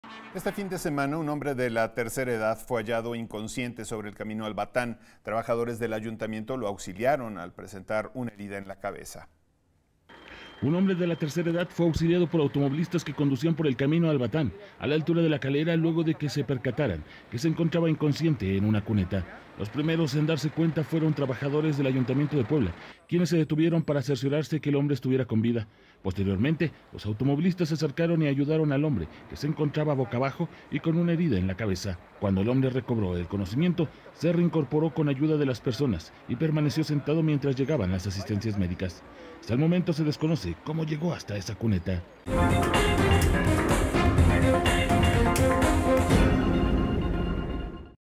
Estaba inconsciente en la calle, la gente lo ayudó
Este fin de semana un hombre de la tercera edad fue hallado inconsciente Camino Al Batan, trabajadores del Ayuntamiento lo auxiliaron